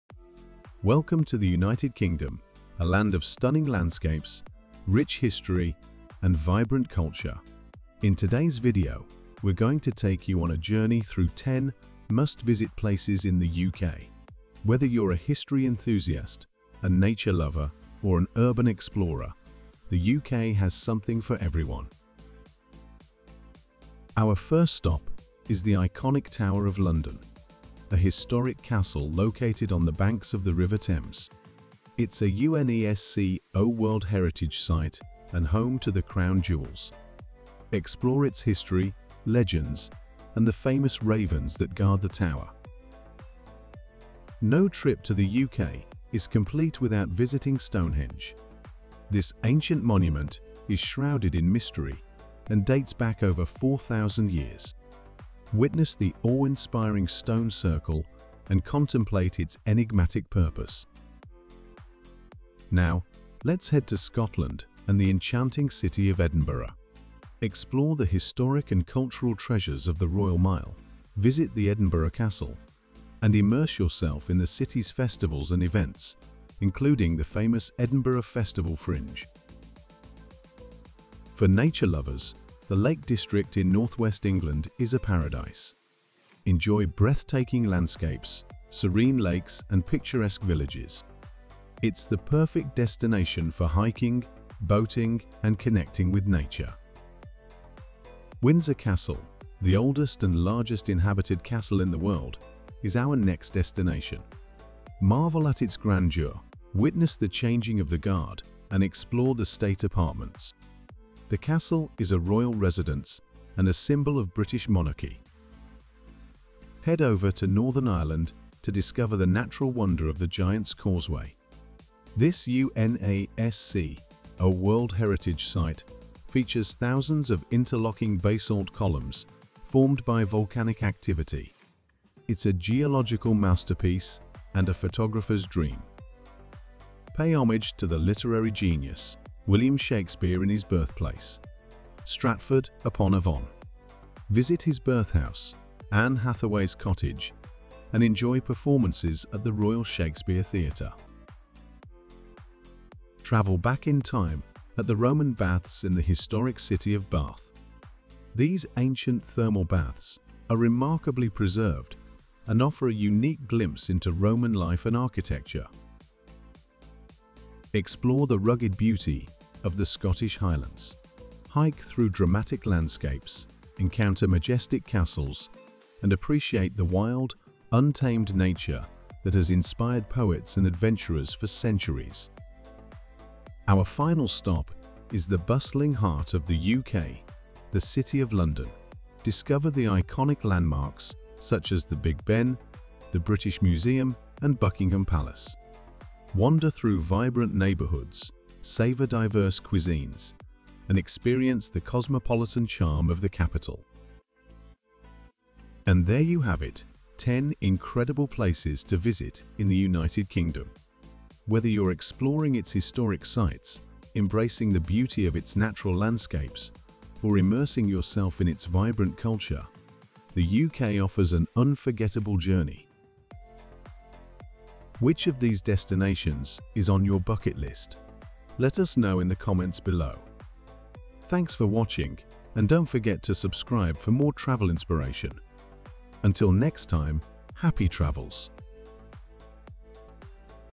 10 Must-Visit Places in the UK | Explore the United Kingdom's Treasures
10 Must-Visit Places in the UK | Explore the United Kingdom's Treasures

Welcome to Visual Journeys, your gateway to immersive education and exploration. In today's video, we embark on a captivating journey through the United Kingdom, unveiling its 10 must-visit places. From historic landmarks to breathtaking natural wonders, this video offers a comprehensive tour of the UK's finest treasures.

Our voyage commences at the iconic Tower of London, a UNESCO World Heritage Site brimming with legends and the dazzling Crown Jewels. Next, we journey back in time at Stonehenge, a 4,000-year-old enigma that continues to captivate the imagination.

Our adventure takes us to Scotland, where the enchanting city of Edinburgh beckons. Explore the Royal Mile, visit the Edinburgh Castle, and immerse yourself in the city's vibrant festivals, including the renowned Edinburgh Festival Fringe.

For nature enthusiasts, the Lake District awaits with its majestic landscapes, serene lakes, and idyllic villages. It's the ideal destination for hikers, boaters, and those seeking a deep connection with nature.

Windsor Castle, the oldest inhabited castle globally, invites you to marvel at its grandeur, witness the Changing of the Guard, and explore the State Apartments. This is the heart of British monarchy.

Venture to Northern Ireland to witness the natural wonder of the Giant's Causeway, a geological masterpiece formed by volcanic activity, and a paradise for photographers.

In Shakespeare's birthplace, Stratford-upon-Avon, pay homage to the literary genius. Visit his birth house, Anne Hathaway's Cottage, and enjoy performances at the Royal Shakespeare Theatre.

The Roman Baths in Bath offer a journey back in time, providing a unique glimpse into ancient Roman life and architecture, remarkably preserved over the centuries.

Discover the untamed beauty of the Scottish Highlands, where dramatic landscapes and majestic castles await, inspiring poets and adventurers alike.

Our final destination, the heart of the UK, the City of London, showcases iconic landmarks like Big Ben, the British Museum, and Buckingham Palace. Wander through vibrant neighborhoods, savor diverse cuisines, and experience the cosmopolitan charm of the capital.

Join us on this unforgettable journey through the United Kingdom, where history, natural beauty, and vibrant culture intersect.